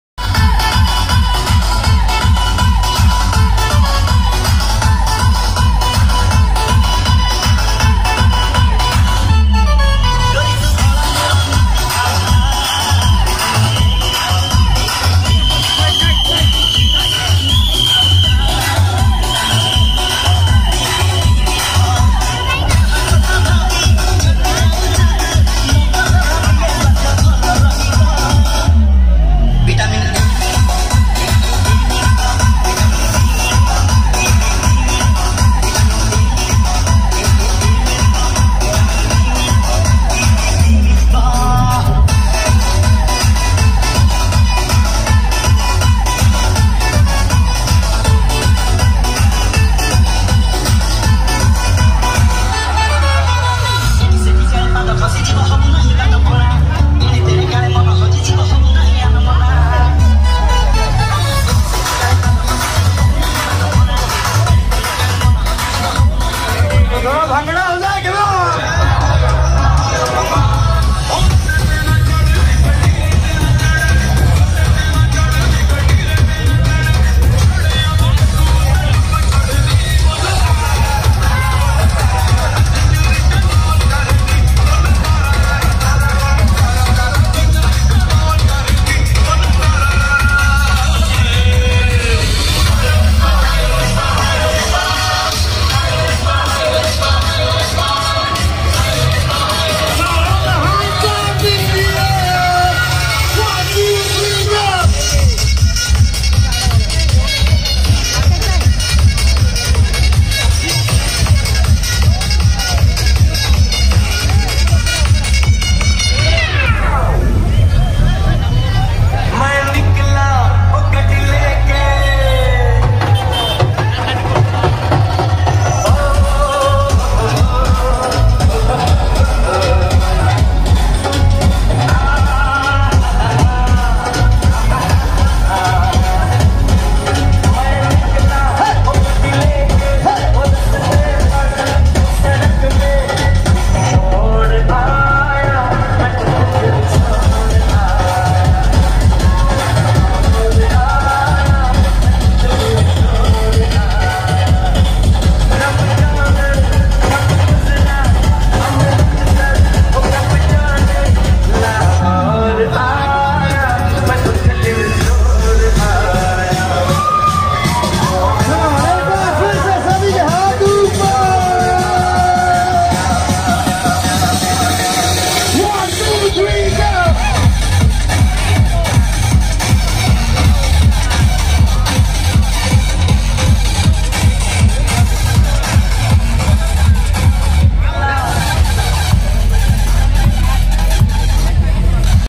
DJ SAI PRO Bhubaneswar Durga Puja Bhasani 2022 | Professional Light & Sound System | By KMS
Hii Friends Welcome to Our Chennel Today tapic  Dj Vlog Video  Is

Hit Likes.

Lots Of ❤️